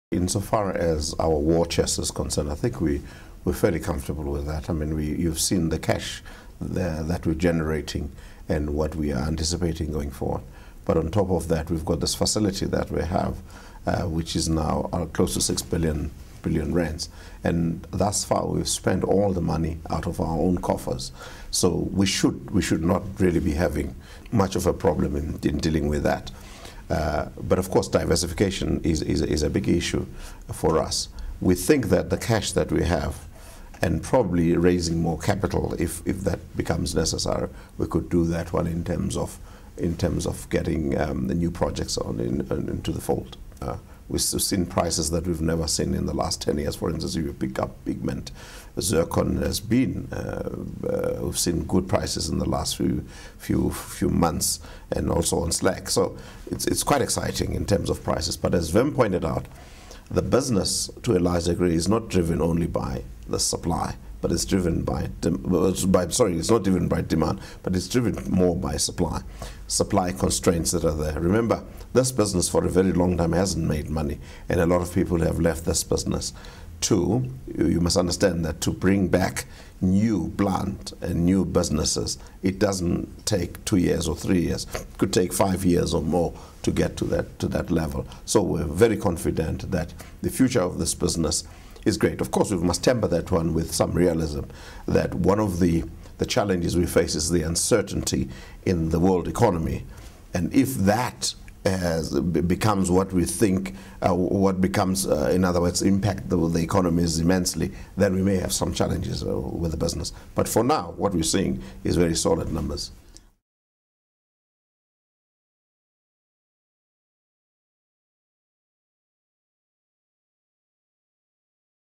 Exxaro to step up capex to R7.1bn next year
JOHANNESBURG (miningweekly.com) -- JSE-listed Exxaro Resources plans to increase its capital expenditure (capex) to R7.1-billion in the 2012 financial year, which would further complement the miner's diversification strategy.